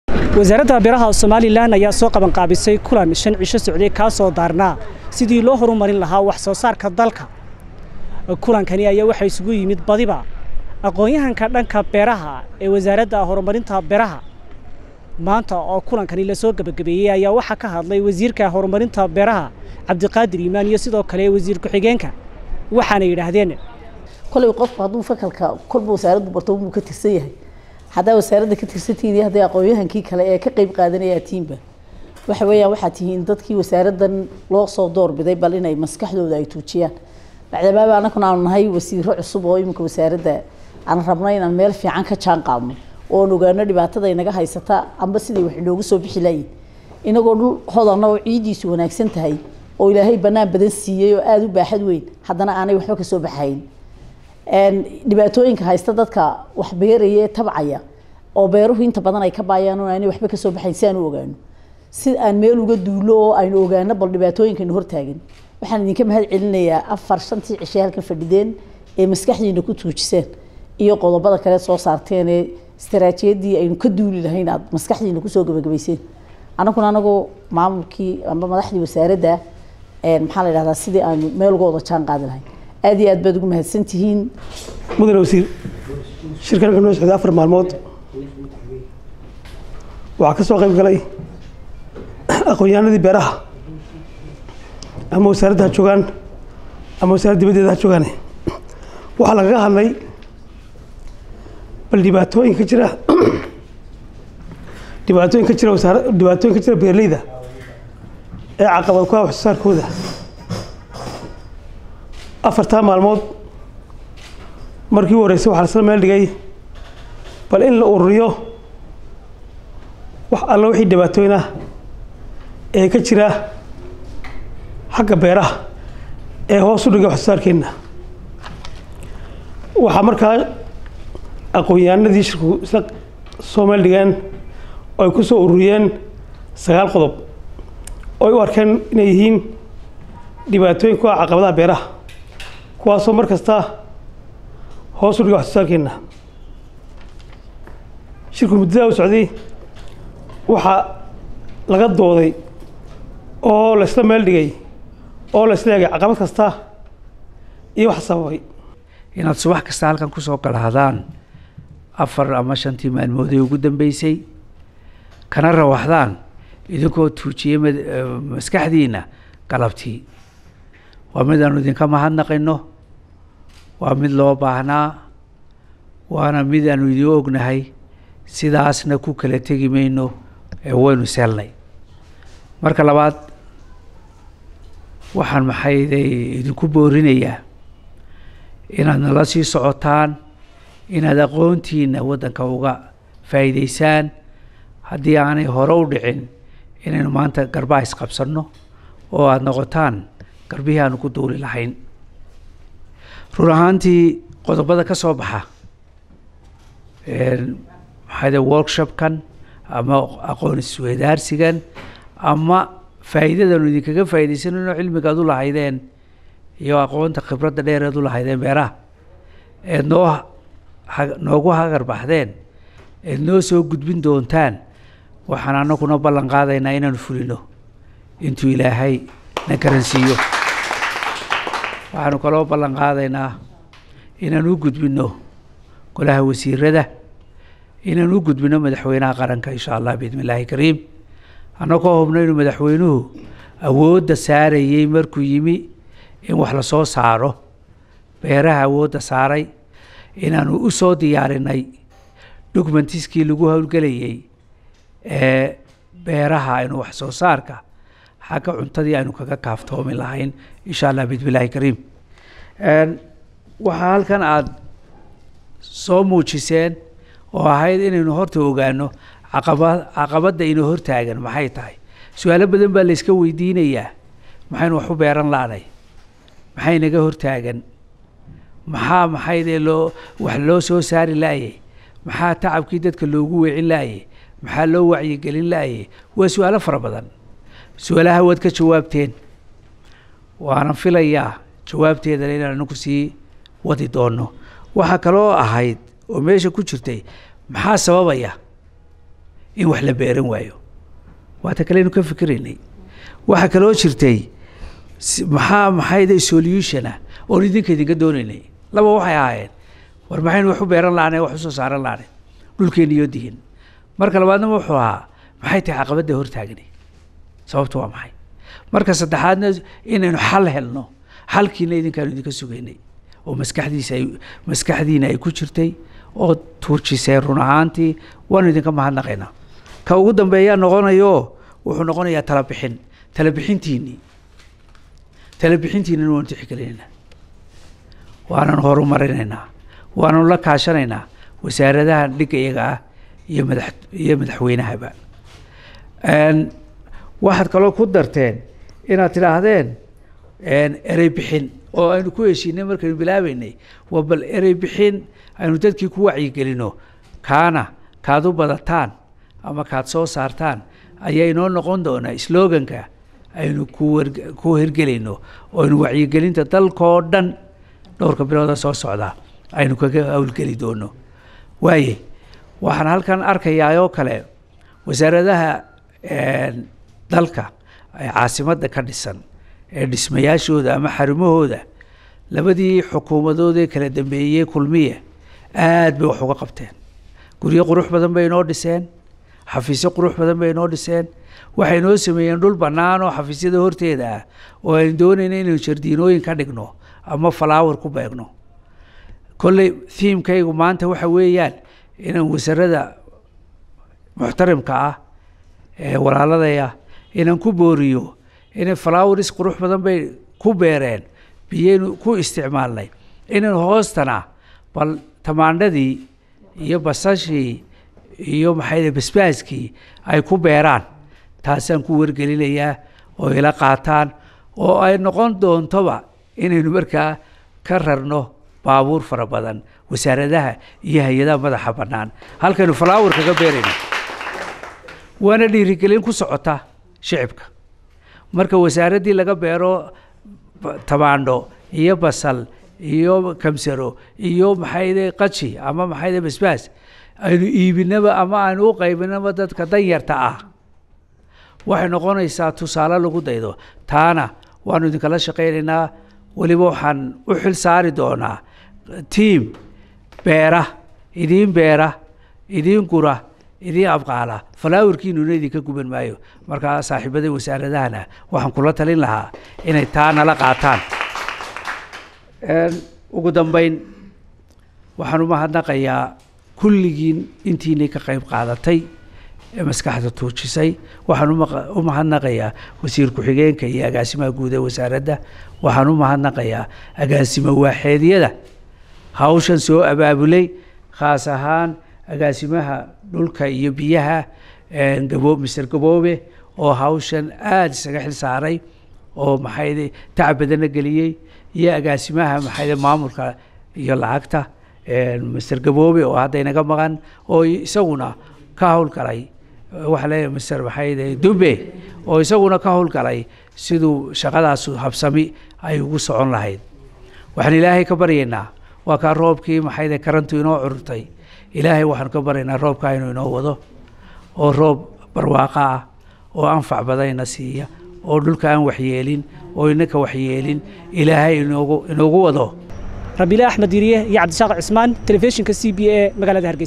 W/Beeraha JSL ayaa soo xidhay kulan lagu lafagurayey sidii loo horumarin lahaa wax-soo-saarka.
Wasaaradda beeraha Soomalilaan oo soo gebagabaysay kulan lagu lafa-gurayey sidii loo horumarin lahaa wax-soo-saarka dalka.